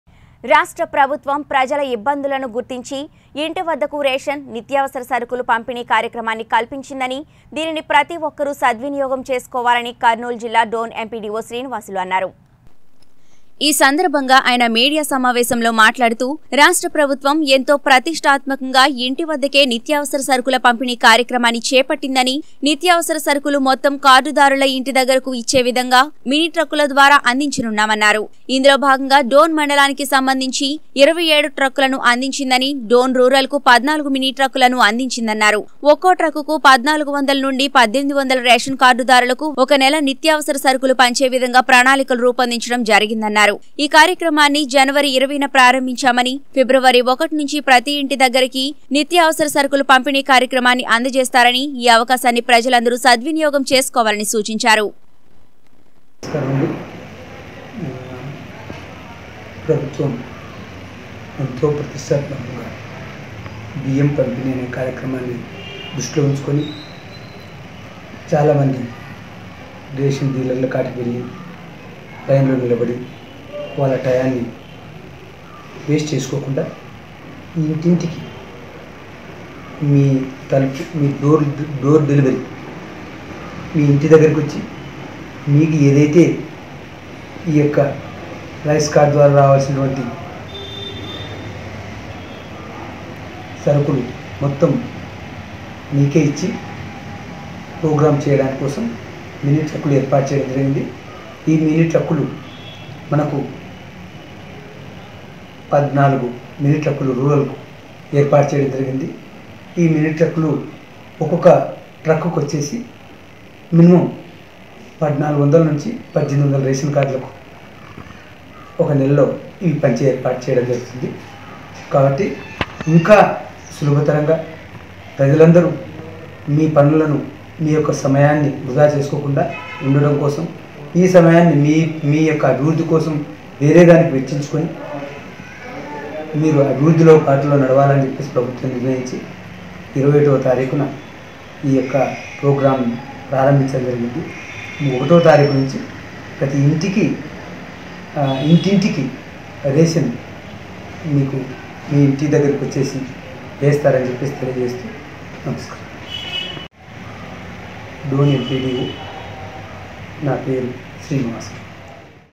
ఇంటి వద్దకు రేషన్, నిత్యావసర సరుకులపంపిణీ ప్రతిఒక్కరూ సద్వినియోగం చేసుకోవాలి - ఎంపీడీవో శ్రీనివాసులు
ఇంటి వద్దకు రేషన్, నిత్యావసర సరుకుల పంపిణీ ప్రతి ఒక్కరూ సద్వినియోగం చేసుకోవాలి కర్నూలు జిల్లా డోన్ ఎంపీడీవో శ్రీనివాసులు | 19 Newstelugu